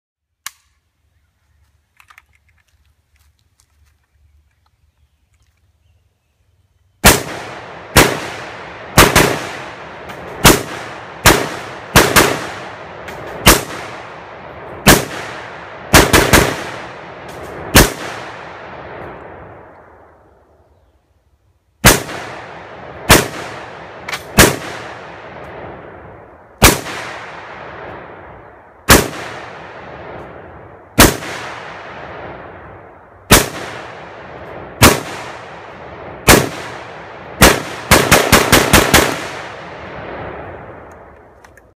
Yugoslavian Zastava M92 PAP SBR 7.62x39 Krink
Just booming the Pap.  Stock is a Romy held on with a Romy rear trunnion.